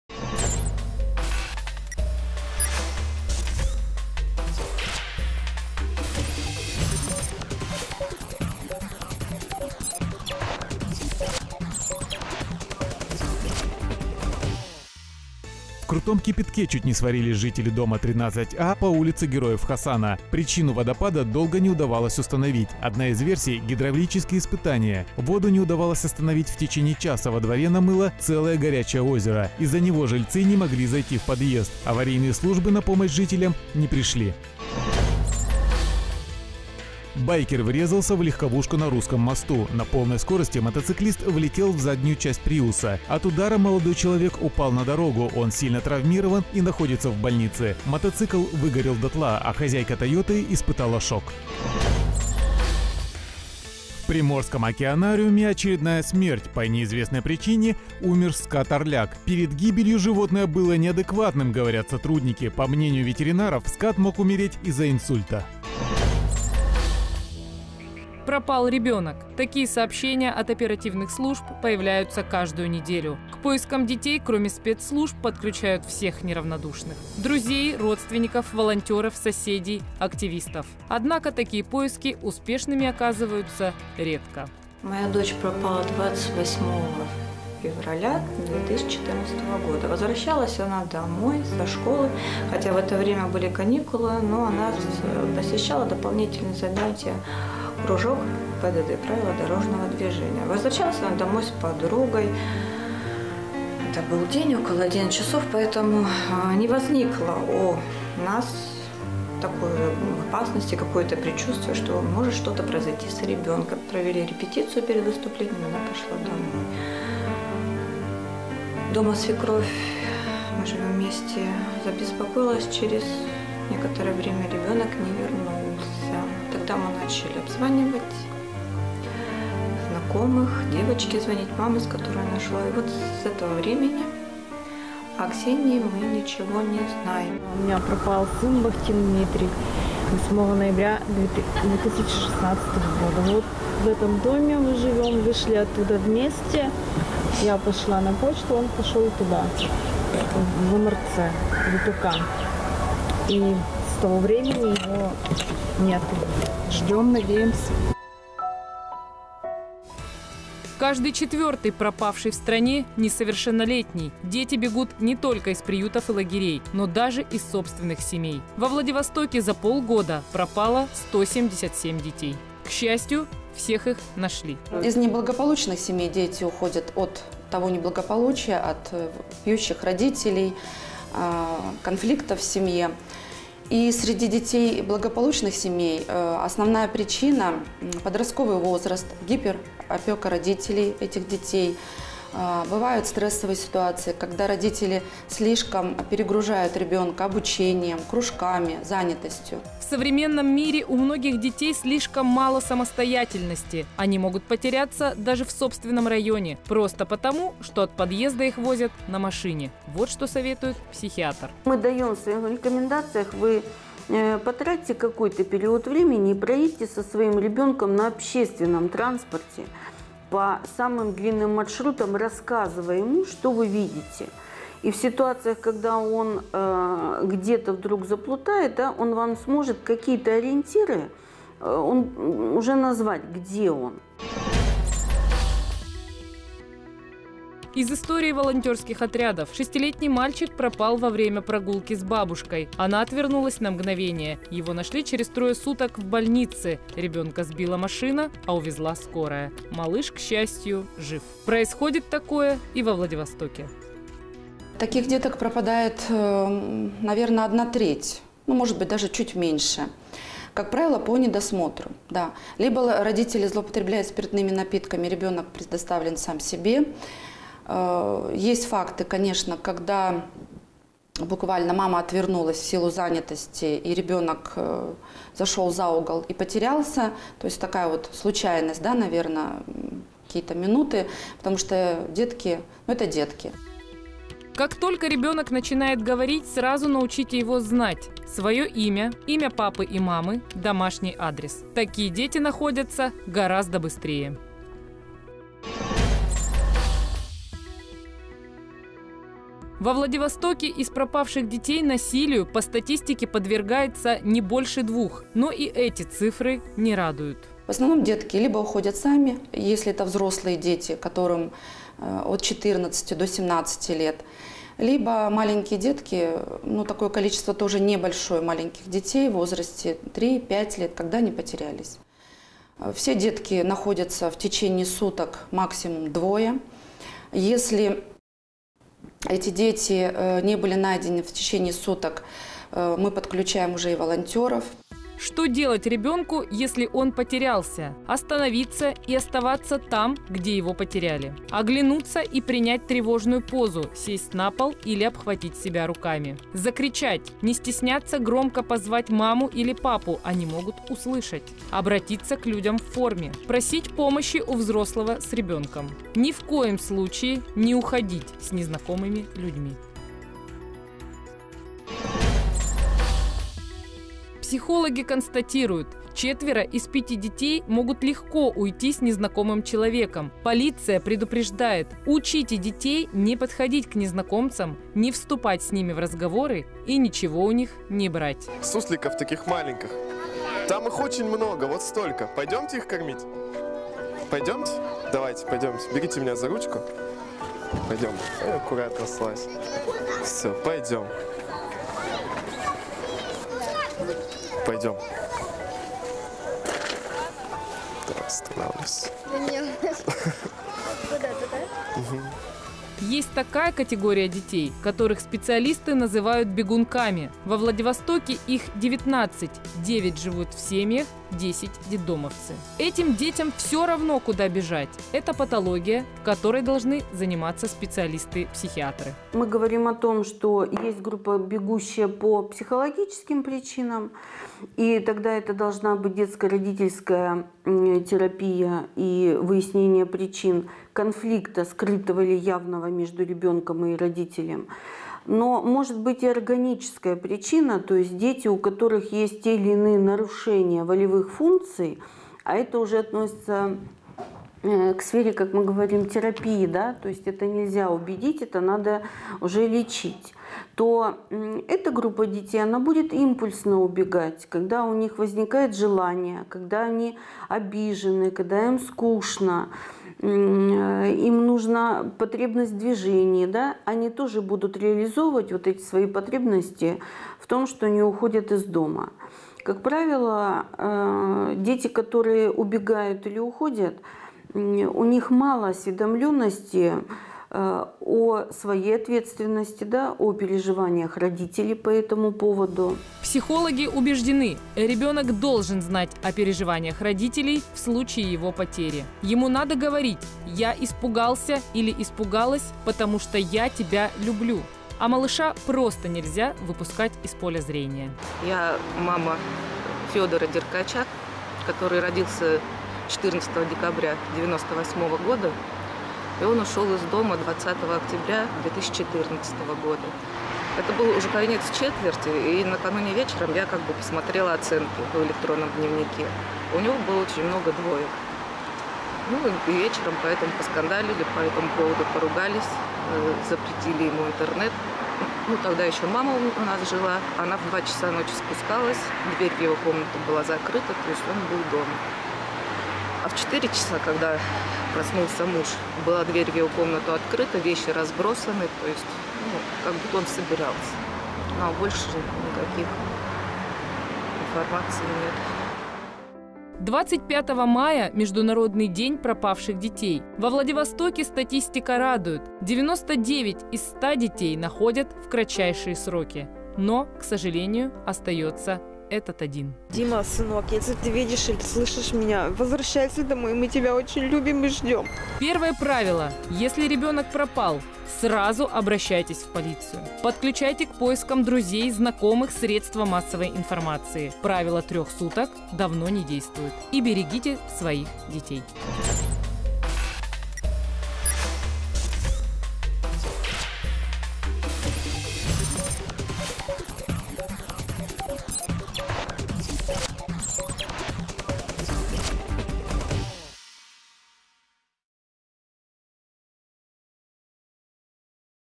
Оперативная съемка. Пропал ребенок
Пропал ребенок. Такие сообщения от оперативных служб появляются каждую неделю. К поискам детей кроме спецслужб подключают всех неравнодушных – друзей, родственников, волонтеров, соседей, активистов. Однако такие поиски успешными оказываются редко.